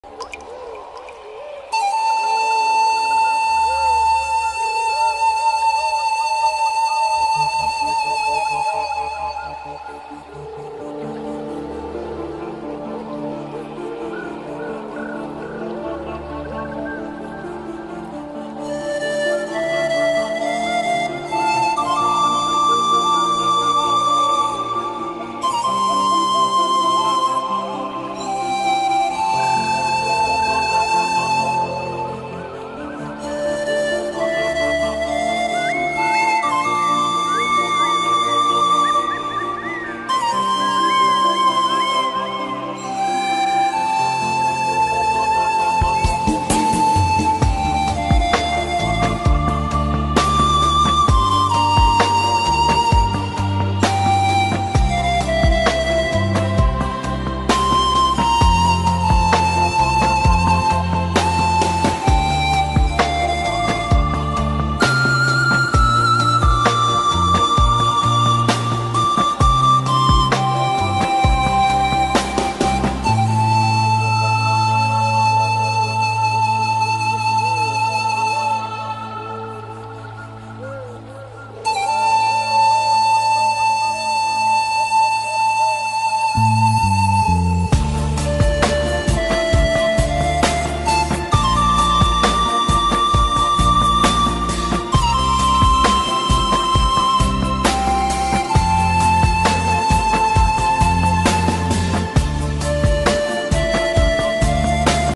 Stars in the Wadi Rum desert
A night in a desert is very beautiful because there are many stars and meteors because there is no light but stars and the moon😍